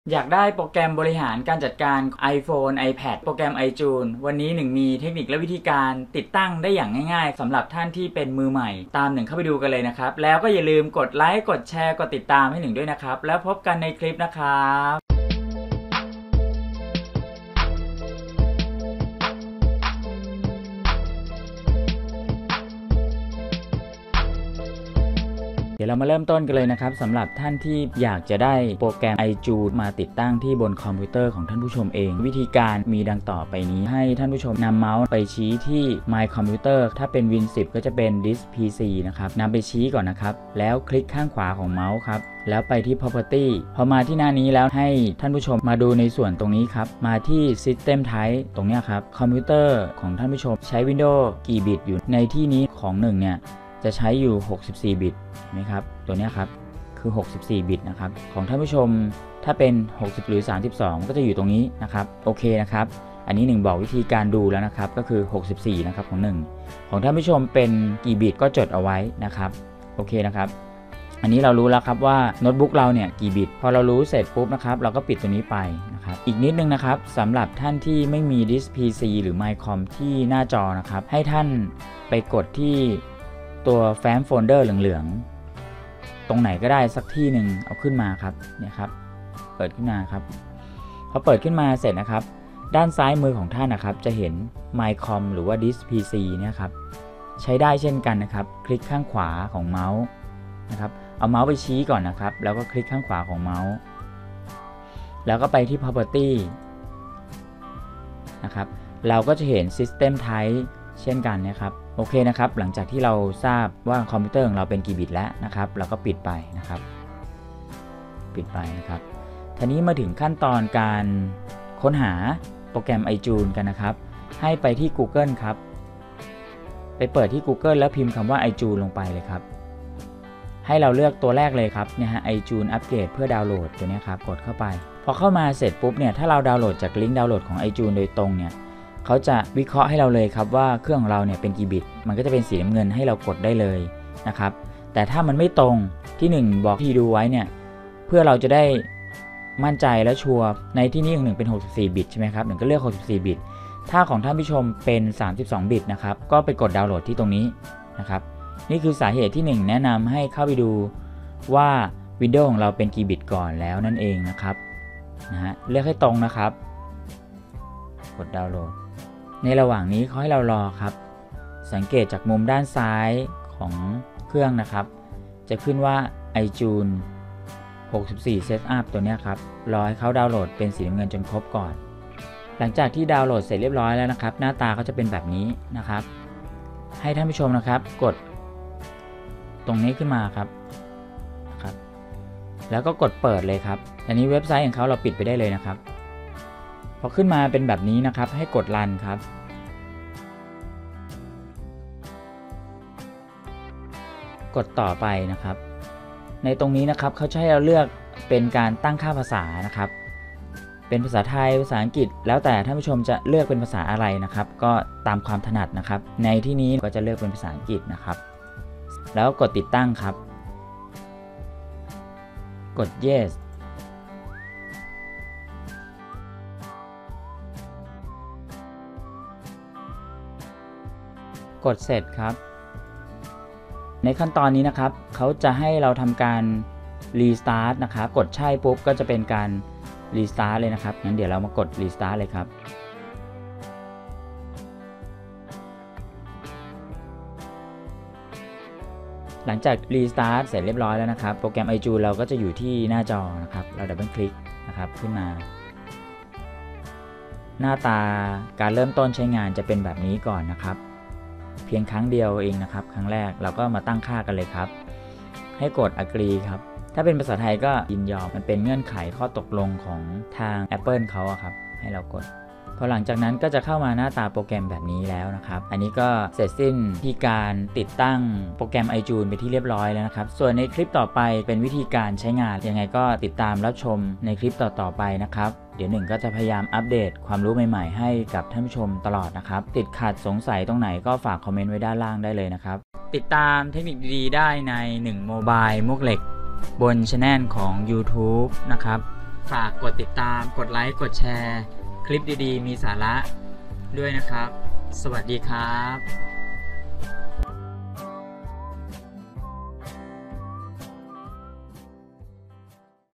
ติดตั้ง itunes ลงคอม ดาวน์โหลด เวอร์ชั่นล่าสุด มือใหม่ อัพเดท2019 l หนึ่งโมบายมวกเหล็ก ครูหนึ่งสอนดี
วิธีติดตั้งโปรแกรม itunes ลงบนคอมพิวเตอร์ สำหรับมือใหม่ ง่ายๆครับ

ติดต่อส่งเครื่องซ่อมได้ที่เฟสบุ๊ค หนึ่งโมบายมวกเหล็ก ฝากข้อความไว้นะครับ

เฟสบุ๊ค ครูหนึ่งสอนดี